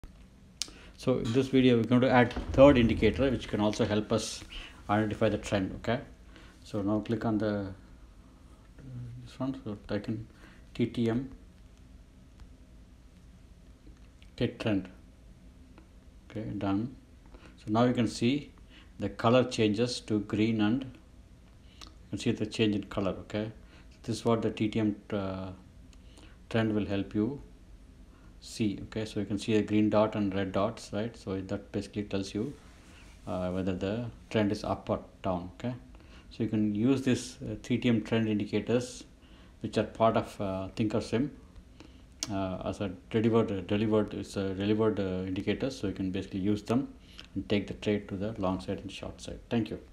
TTM Trend | Easy to Identify the Trend of the Market |Thinkorswim
TTM Trend

TTM Trend

TTM_Trend
Description
The TTM_Trend study is intended to indicate the true direction of the trend replacing false signals with true ones.

The TTM_Trend study converts the normal chart to the view where irregularities are eliminated. In TTM_Trend, a bar is shown as bearish when the Average Price has closed in the lower 50% price range of the input-defined number of previous bars. If the Average Price has otherwise closed in the upper 50% range, the bar is shown as bullish.